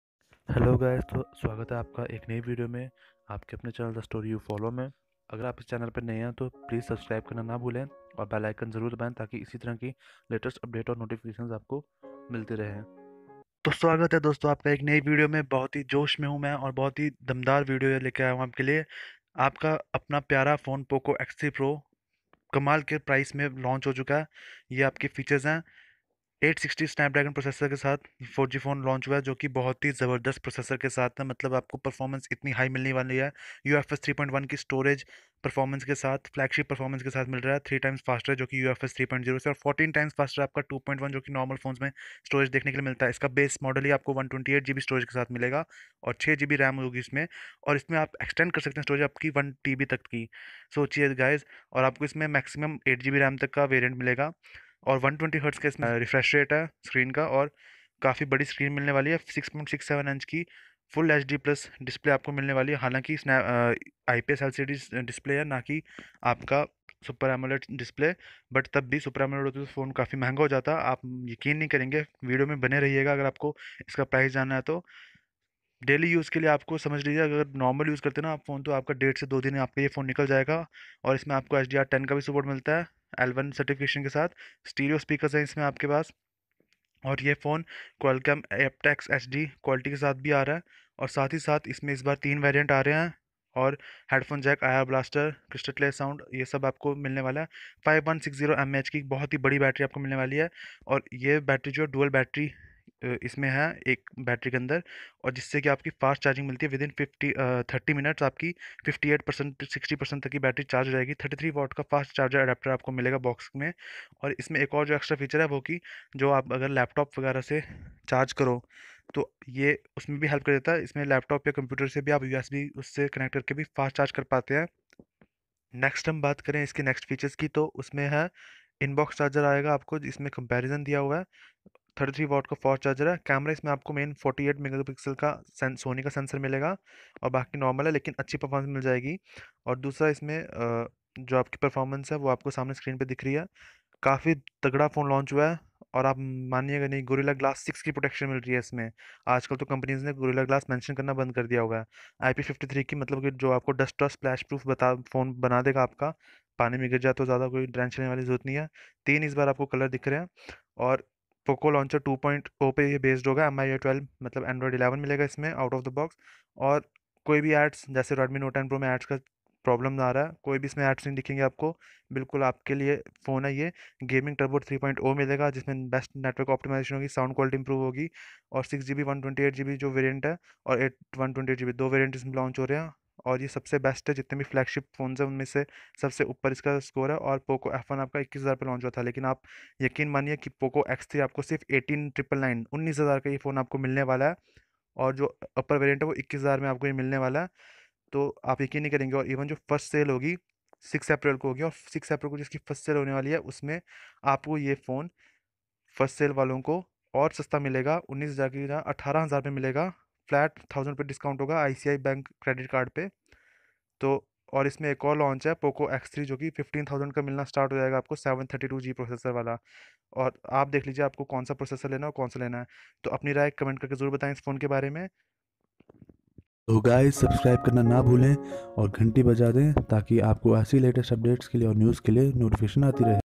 Poco x3 pro price out | Launched at just rs18999 only | by the_story_u_follow
Poco x3 pro is launched today | poco x3 pro launchprice 18999 base variant | Really a flagship killer just @18999 also earlybird offer gives you at 17999
if you have a budget of 20k . This is the phone for you guys...even better than Redmi note 10 series if not taken for camera
Go grab it

the_story_u_follow
thestoryufollow